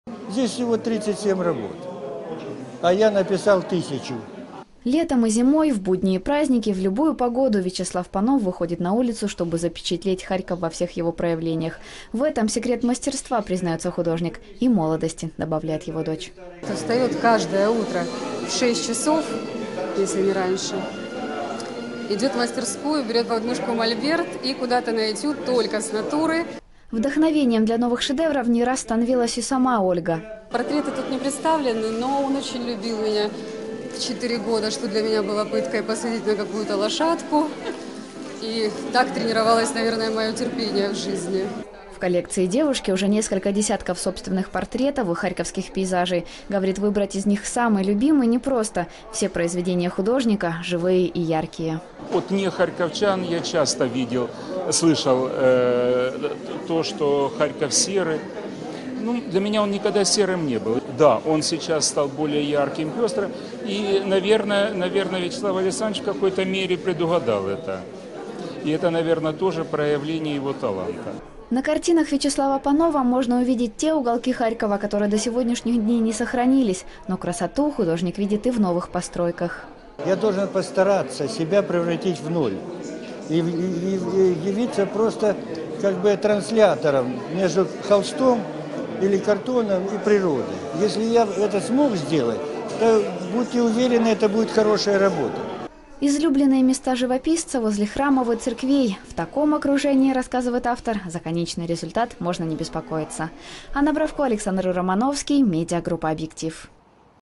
В Харькове открылась выставка городских пейзажей
- Харьков в прошлом и нынешний. Полвека художник рисует каждый день из жизни города - Десятки живописных уголков Харькова собрали под одной крышей. В галерее «Мистецтво Слобожанщини» открылась выставка картин Вячеслава Панова